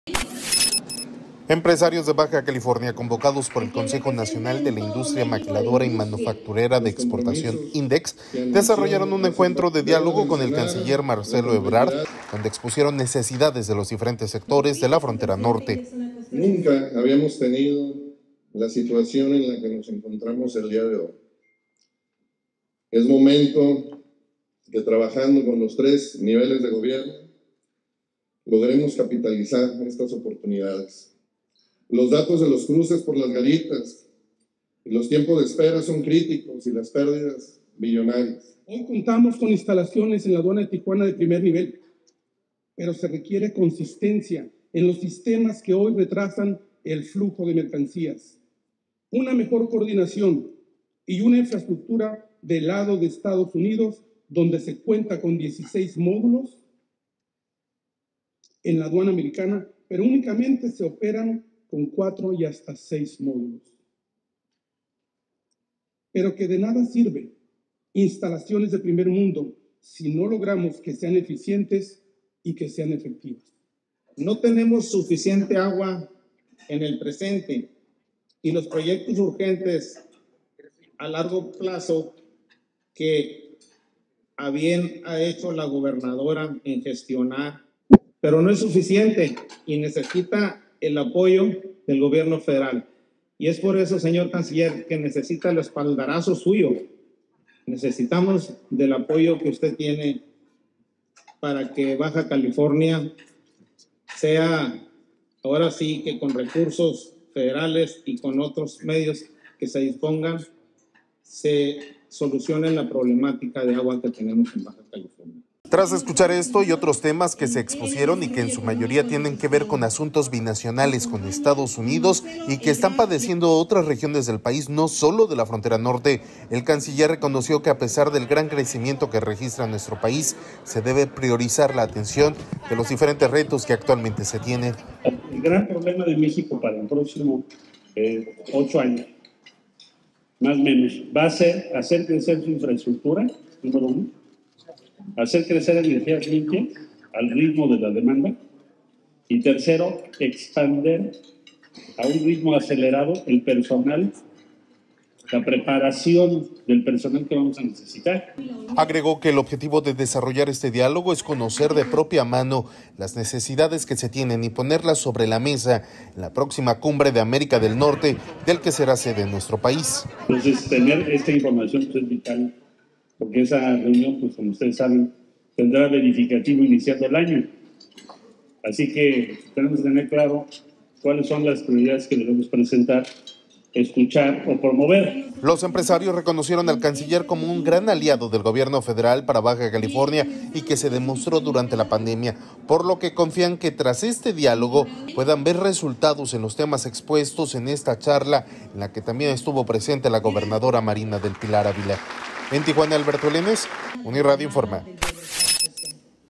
Ebrard expondrá necesidades de BC en Cumbre de Norteamérica
- “Vengo a conocer sus necesidades para exponerlas en la Cumbre de América del Norte”: Ebrard.
- Empresarios piden al canciller la agilización en los tiempos de cruce a los productos de exportación a EU.
- El Secretario de Relaciones Exteriores tuvo una reunión con integrantes de Index y la industria exportadora de Tijuana para preparar la Cumbre de América del Norte